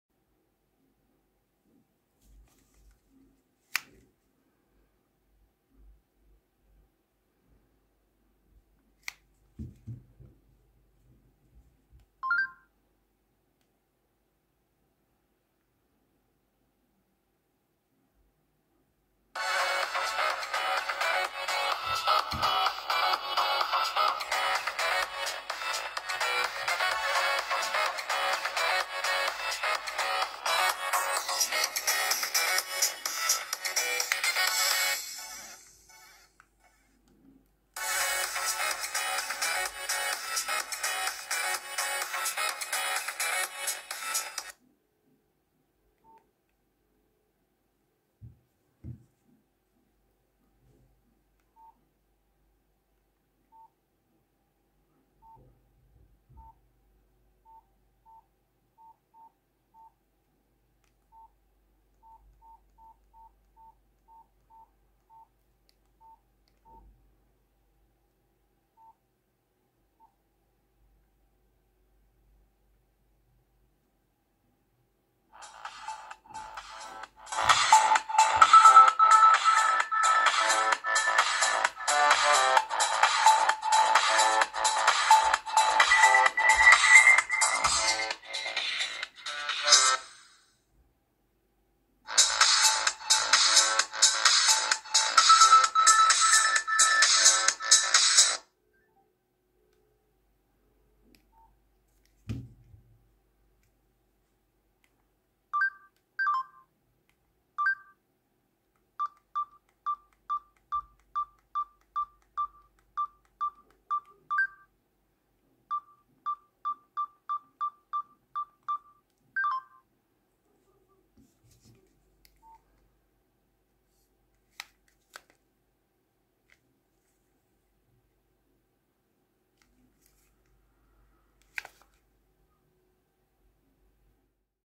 Incoming call & Outgoing call at the Same Time Nokia 7610 + Samsung C6112
Incoming call Nokia 7610 supernova
Incoming call Samsung GT-C6112